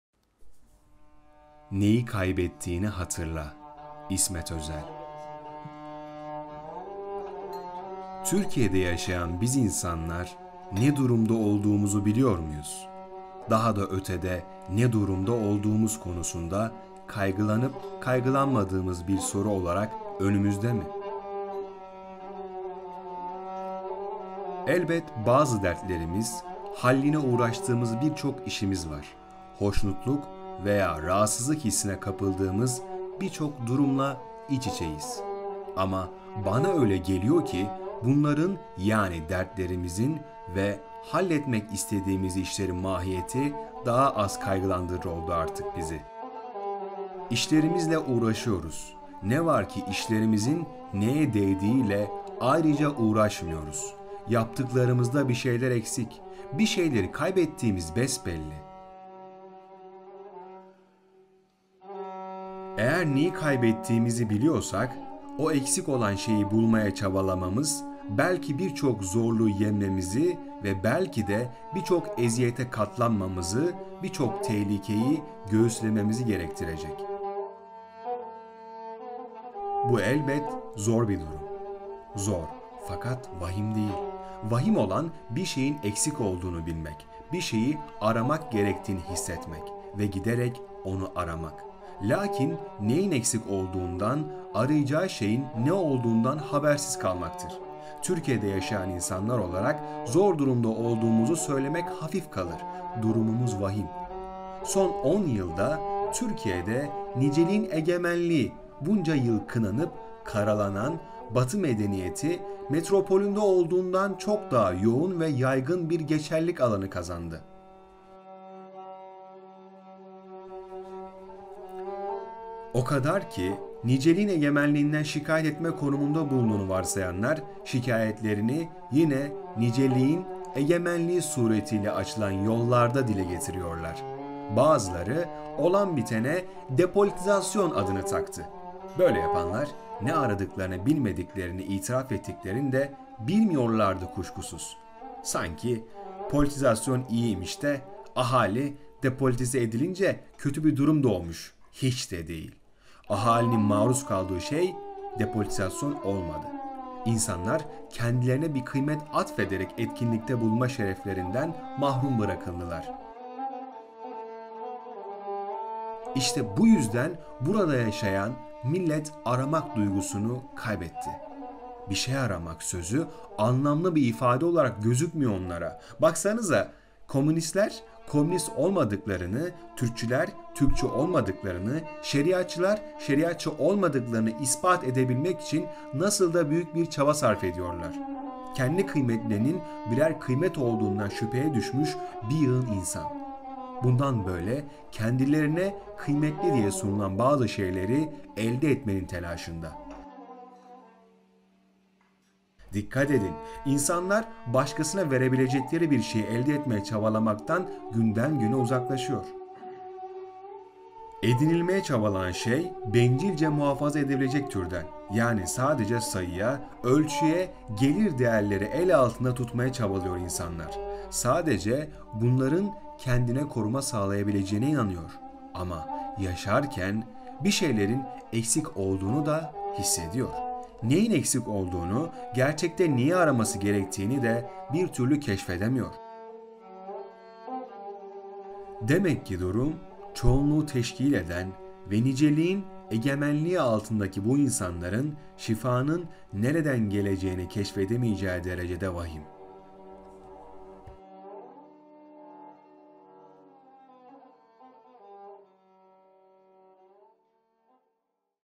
İsmet Özel - Neyi Kaybettiğini Hatırla
Neyi Kaybettiğini Hatırla kitabındaki ''Neyi Kaybettiğini Hatırla'' bölümünün seslendirilmesi.

hep şiirle kalın..

istek,destek,öneri..

Türkiye'de yaşayan biz insanlar ne durumda olduğumuzu biliyor muyuz? Daha da ötede "ne durumda olduğumuz" konusunda kaygılanıp kaygılanmadığımız bir soru olarak önümüzde mi? Elbet bazı dertlerimiz, halline uğraştığımız birçok işimiz var. Hoşnutluk veya rahatsızlık hissine kapıldığımız birçok durumla içiçeyiz. Ama bana öyle geliyor ki, bunların, yani dertlerimizin ve halletmek istediğimiz işlerin mahiyeti daha az kaygılandırır oldu artık bizi. İşlerimizle uğraşıyoruz; ne var ki işlerimizin neye değdiğiyle ayrıca uğraşmıyoruz. Yaptıklarımızda bir şeyler eksik. Birşeyleri kaybettiğimiz besbelli.
Eğer neyi kaybettiğimizi biliyorsak o eksik olan şeyi bulmaya çabalamamız belki birçok zorluğu yenmemizi ve belki de bir çok eziyete katlanmamızı, bir çok tehlikeyi göğüslememizi gerektirecek. Bu elbet zor bir durum. Zor, fakat vahim değil. Vahim olan bir şeyin eksik olduğunu bilmek, bir şeyi aramak gerektiğini hissetmek ve giderek onu aramak; lâkin neyin eksik olduğundan, arayacağı şeyin ne olduğundan habersiz kalmaktır. Türkiye'de yaşayan insanlar olarak zor durumda olduğumuzu söylemek hafif kalır; durumumuz vahim.
Son on yılda Türkiye'de "niceliğin egemenliği" bunca yıl kınanıp karalanan Batı medeniyeti metropolünde olduğundan çok daha yoğun ve yaygın bir geçerlilik alanı kazandı. O kadar ki niceliğin egemenliğinden şikayet etme konumunda bulunduğunu varsayanlar şikayetlerini yine niceliğin egemenliği suretiyle açılan yollarda dile getiriyorlar. Bazıları olan bitene depolitizasyon adını taktı. Böyle yapanlar ne aradıklarını bilmediklerini itiraf ettiklerini de bilmiyorlardı kuşkusuz. Sanki politizasyon iyi imiş de, ahali depolitize edilince kötü bir durum doğmuş! Hiç de değil. Ahalinin maruz kaldığı şey depolitizasyon olmadı. İnsanlar kendilerine bir kıymet atfederek etkinlikte bulunma şereflerinden mahrum bırakıldılar. İşte bu yüzden burada yaşayan millet aramak duygusunu kaybetti. Bir şey aramak sözü anlamlı bir ifade olarak gözükmüyor onlara. Baksanıza: Komünistler komünist olmadıklarını, Türkçüler Türkçü olmadıklarını, Şeriatçılar Şeriatçı olmadıklarını ispat edebilmek için nasıl da büyük bir çaba sarfediyorlar. Kendi kıymetlerinin birer kıymet olduğundan şüpheye düşmüş bir yığın insan, bundan böyle kendilerine kıymetli diye sunulan bazı şeyleri elde etmenin telaşında.
Dikkat edin: İnsanlar başkasına verebilecekleri bir şeyi elde etmeye çabalamaktan günden güne uzaklaşıyor. Edinilmeye çabalanan şey bencilce muhafaza edilebilecek türden. Yani sadece sayıya, ölçüye gelir değerleri el altında tutmaya çabalıyor insanlar. Sadece bunların kendine koruma sağlayabileceğine inanıyor. Ama yaşarken birşeylerin eksikaraması gerektiğini de bir türlü keşfedemiyor. Demek ki durum çoğunluğu teşkil eden ve niceliğin egemenliği altındaki bu insanların şifanın nereden geleceğini keşfedemeyeceği derecede vahim olduğunu da hissediyor. Neyin eksik olduğunu, gerçekte neyi araması gerektiğini de bir türlü keşfedemiyor. Demek ki durum çoğunluğu teşkil eden ve niceliğin egemenliği altındaki bu insanların şifanın nereden geleceğini keşfedemeyeceği derecede vahim.

Oynatma Listeleri